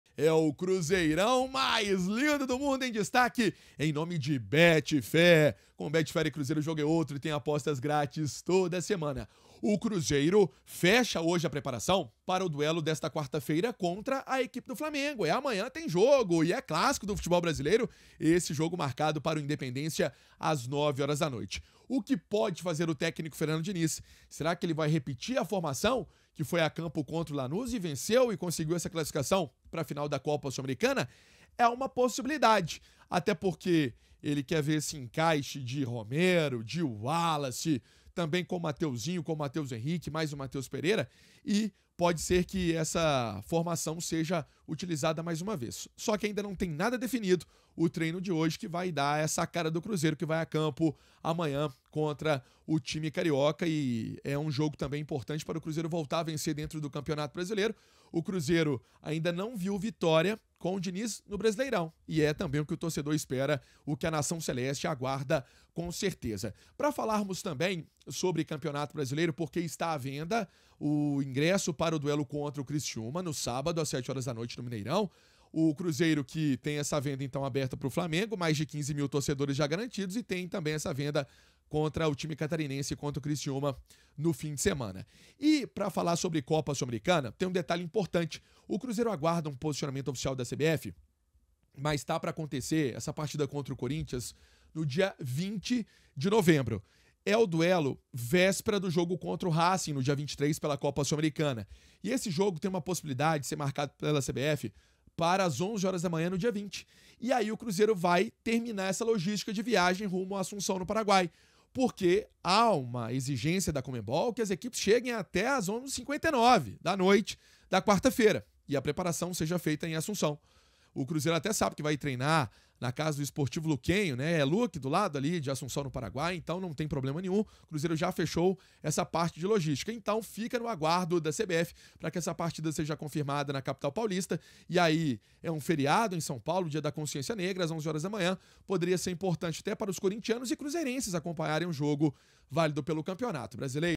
DINIZ AJUSTA O TIME PARA VOLTAR A VENCER NO BRASILEIRÃO | PRIMEIRAS DO CRUZEIRO
O Cruzeiro encara o Flamengo nesta quarta-feira (6) e o técnico Fernando Diniz define hoje o time que entra em campo para conquistar sua primeira vitória no comando pelo Campeonato Brasileiro e voltar a vencer na competição. Confira as primeiras informações com o repórter Emerson Pancieri.